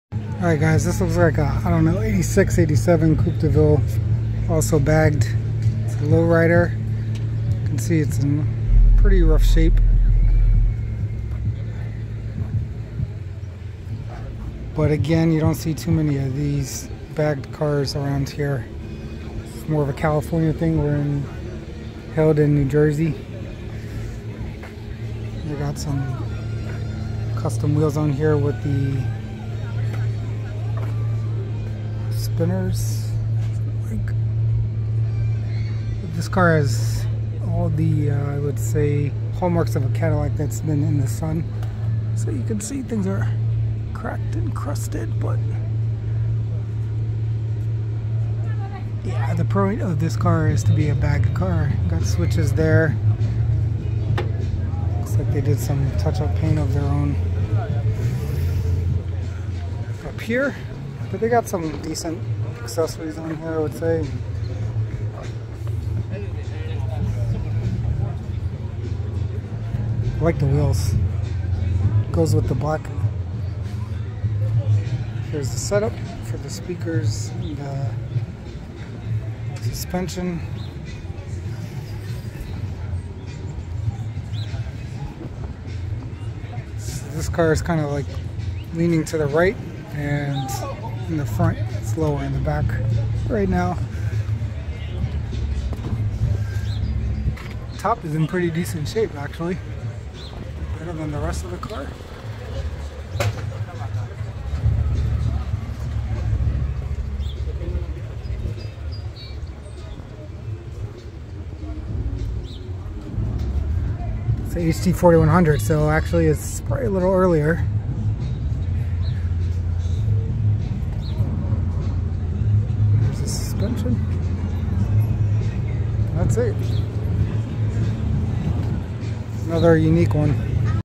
🛄 1982-1987 Cadillac Coupe deVille Lac Lowrider Hydraulics Black on Red
Mid 80s hydraulic Caddy with custom wheels. Daily Driver with an awesome color combo and some DIY finishes. Rare find in NJ!